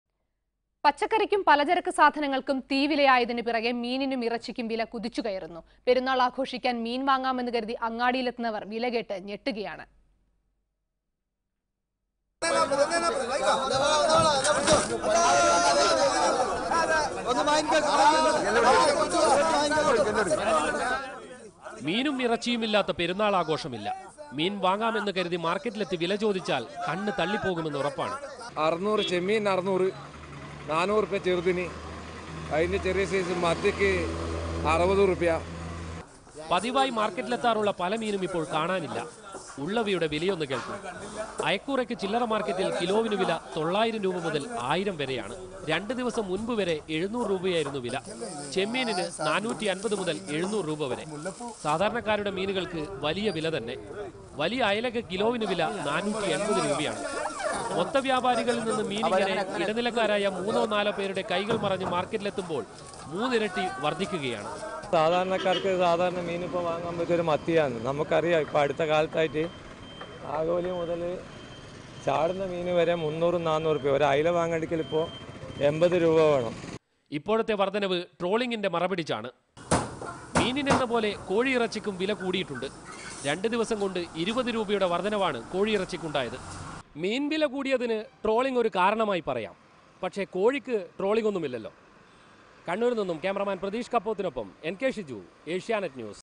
Non-veg hits the roof : പെരുന്നാള്‍ ആഘോഷം പൊള്ളും
The price of chicken and fish market hits the roof.